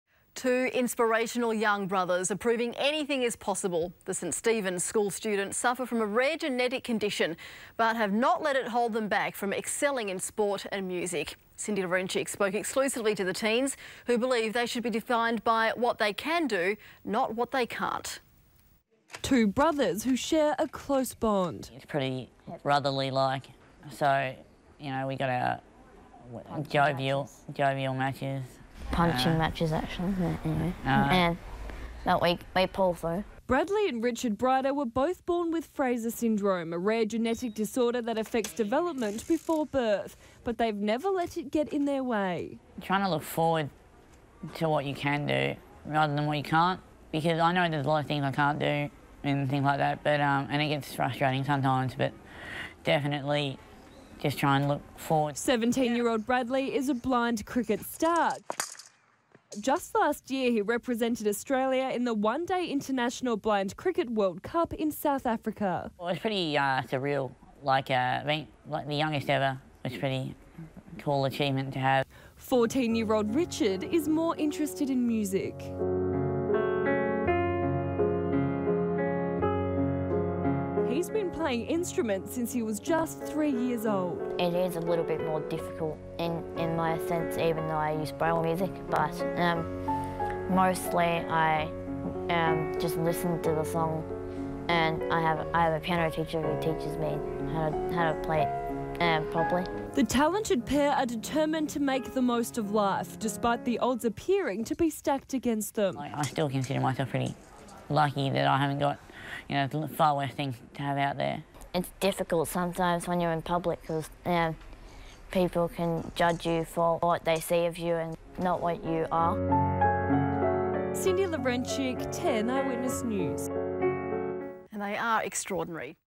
Brave Brothers
This is the heart-warming story of two brave St Stephen’s School brothers. Bradley and Richard Brider have a rare genetic disorder, but that isn’t stopping them from chasing their dreams. This aired on Channel 10 on Friday, August 14, 2015.